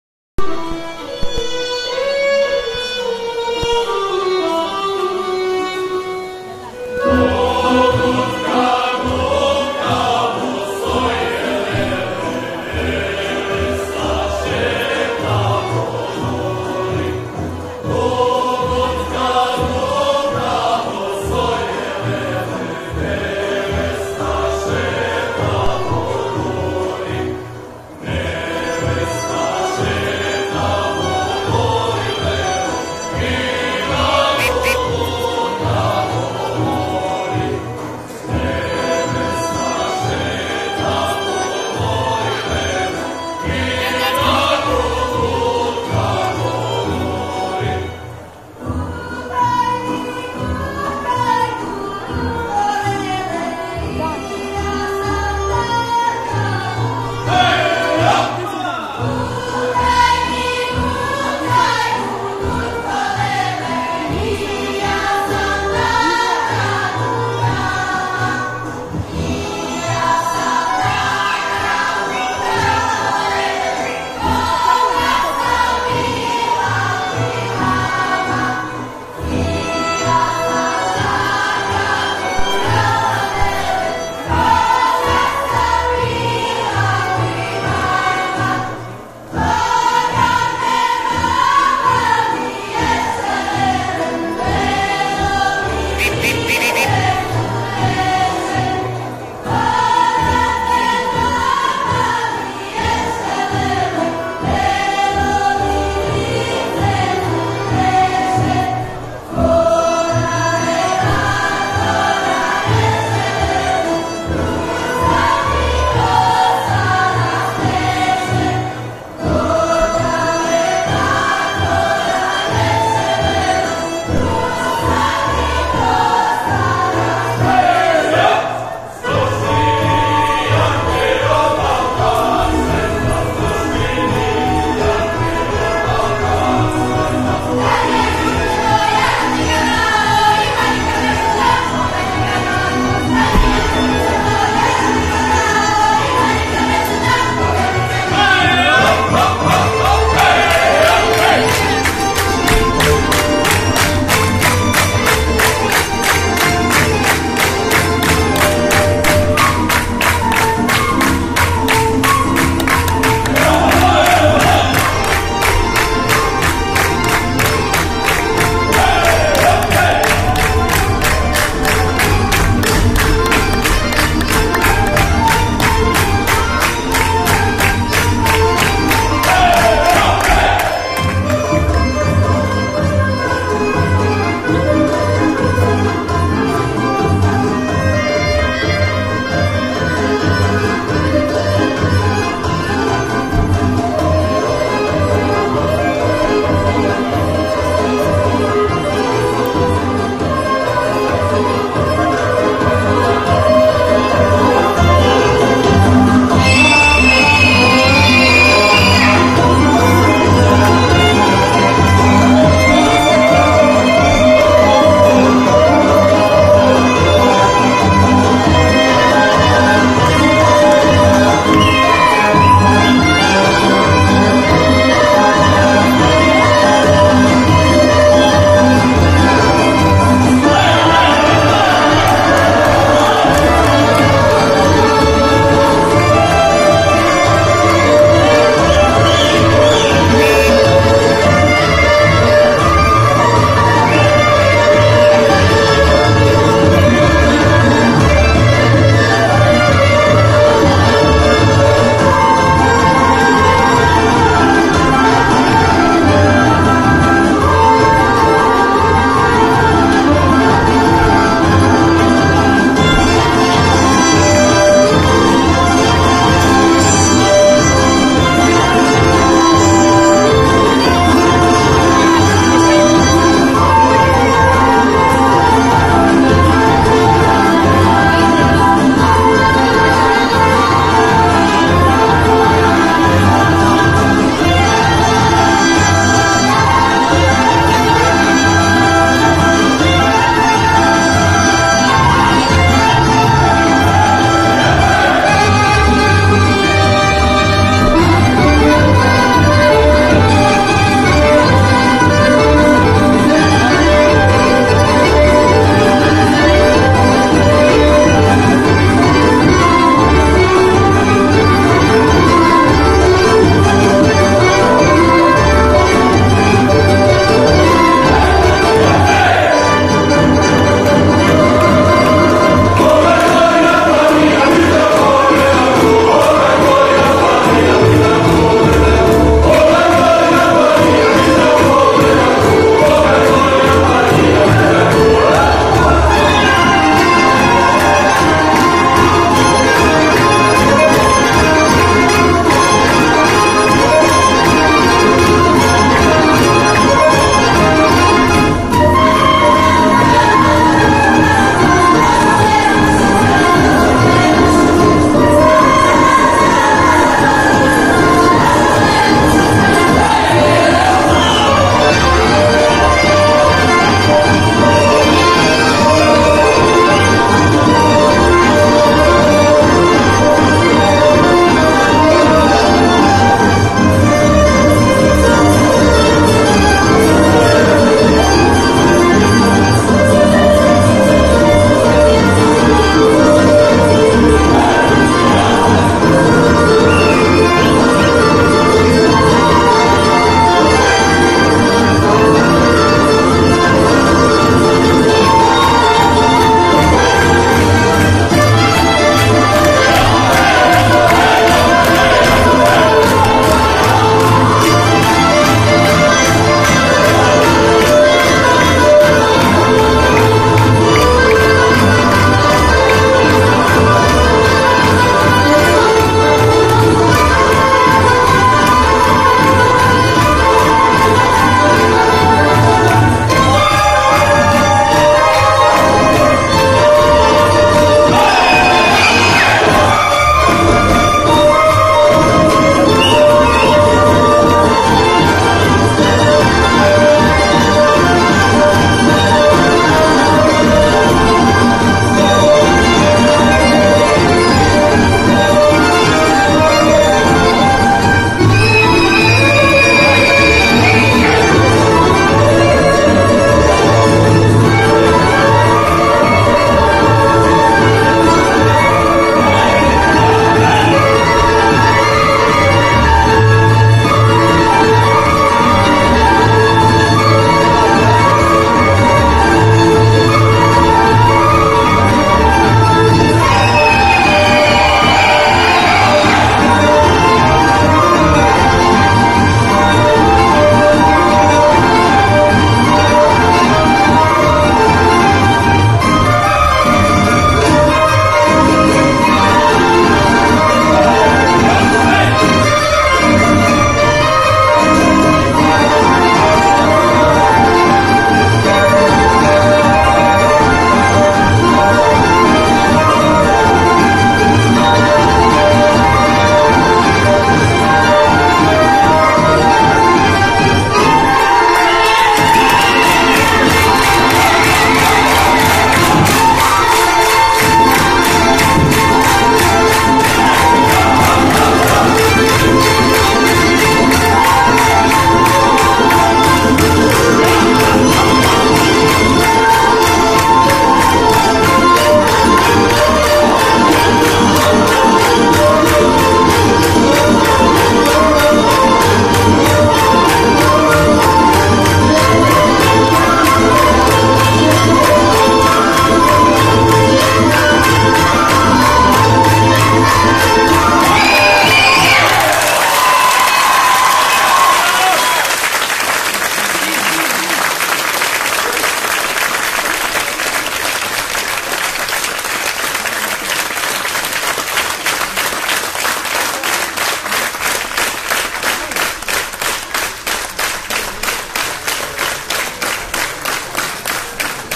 СПКД Просвјета Прњавор - Игре из Босилеградског крајишта (годишњи концерт 22.12.2018. год.)
Игре из Босилеградског крајишта у извођењу Извођачког ансамбла СПКД Просвјета из Прњавора.

Умјетнички руководилац: Бранко Милишић